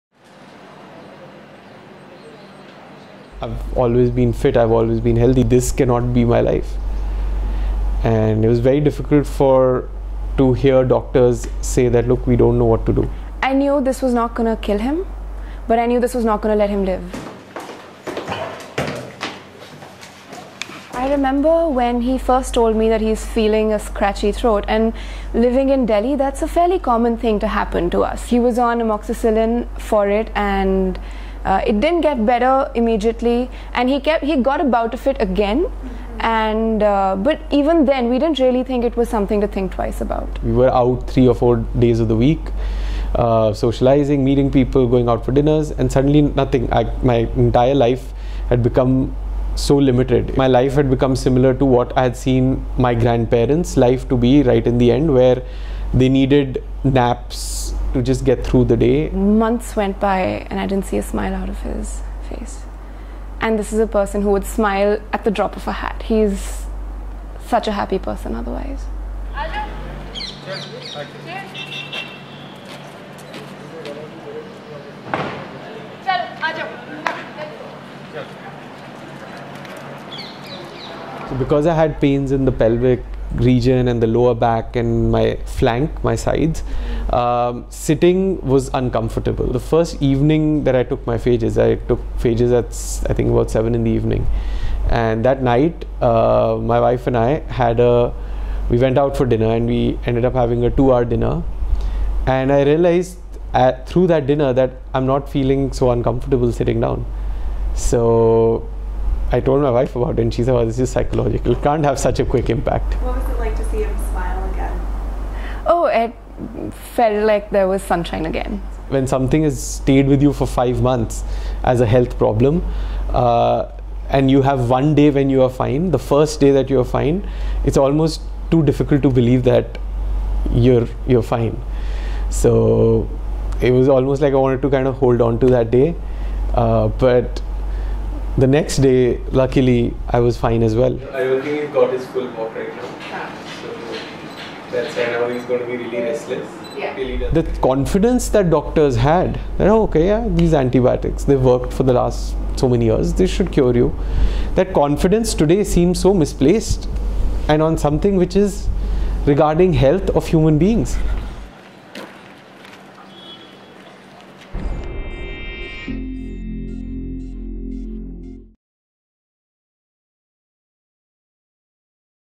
New Delhi man turns to century-old treatment to fight superbug
Pranav Johri paid thousands for phage therapy, which uses natural bacteria-eaters, after five rounds of antibiotics made his prostate issue even worse. A growing number of bacterial infections have become resistant to antibiotics. (June 30, 2018)